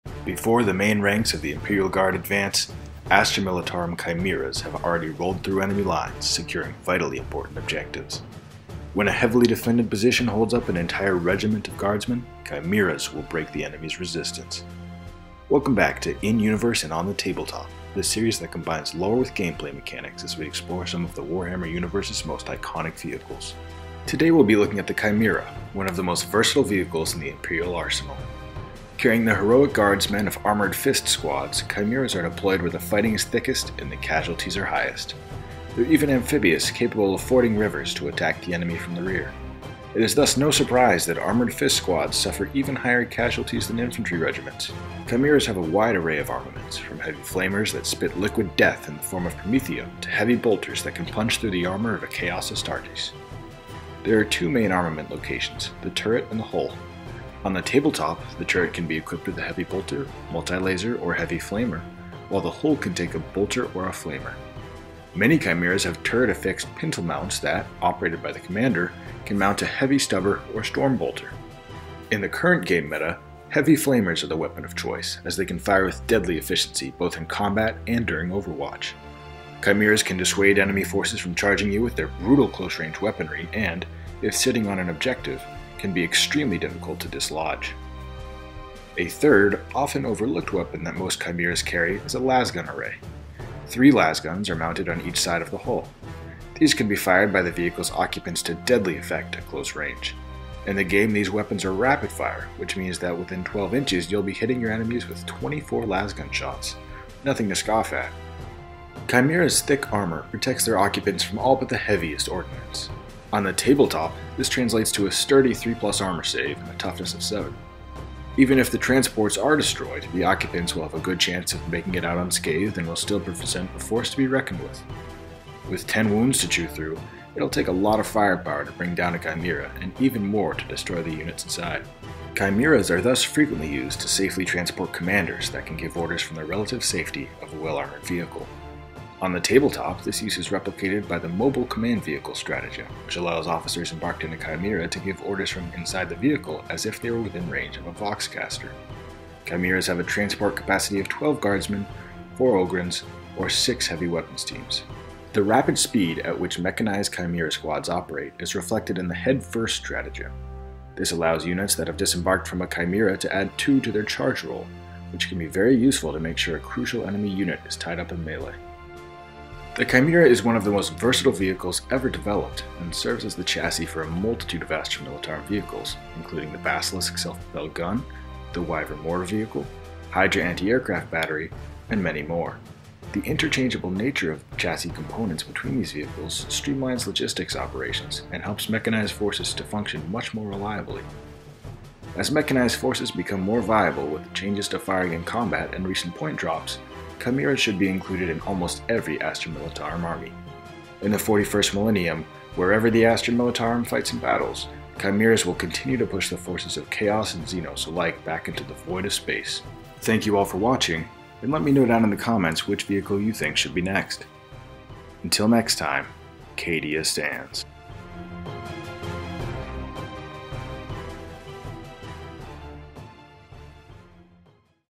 Chimera Troop Transport: Warhammer 40K In Lore and On the Tabletop
Welcome back to “In Universe and on the Tabletop”. Today we’ll be looking at the Chimera, one of the most versatile vehicles in the Imperial arsenal. Carrying the heroic guardsmen of Armored Fist squads, Chimeras are deployed where the fighting is thickest and the casualties are highest.
What are these unique vehicles and how do they perform in Warhammer 40,000?